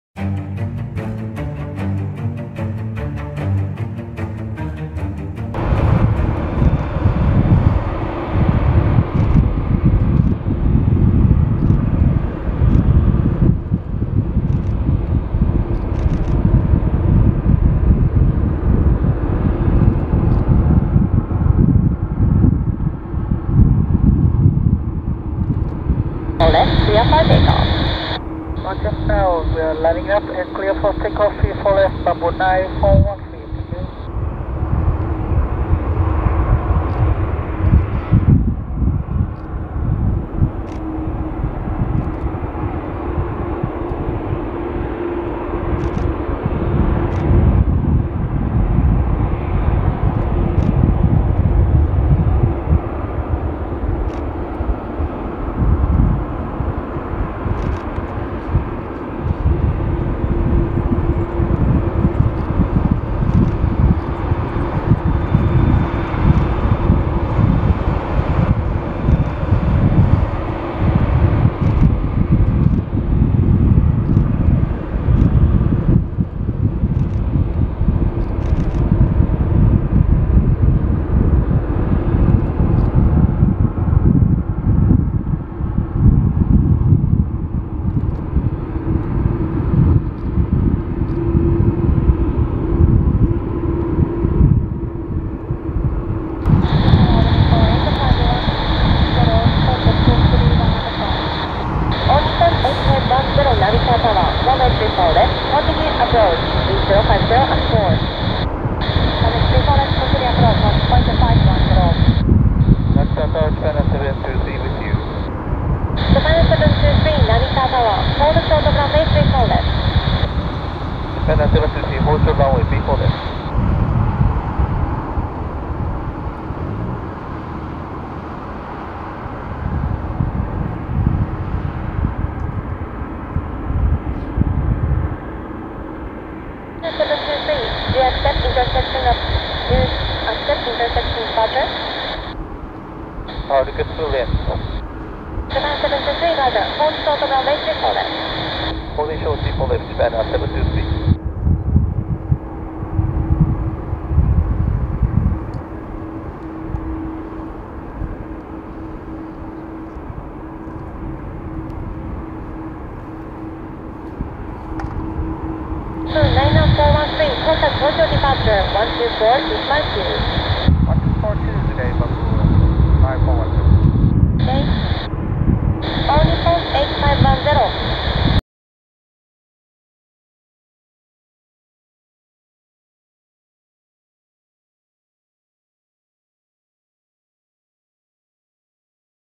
✈✈[RJAA]成田空港 バンブー・エアウエイズ「ハロン湾(Ha Long Bay)」Bamboo Airways Boeing 787-9 VN-A819 Narita RWY34L
ベトナムの航空会社　Bamboo Airways＠kusuyama100
バンブー・エアウェイズBamboo Airways
Boeing 787-9
VN-A819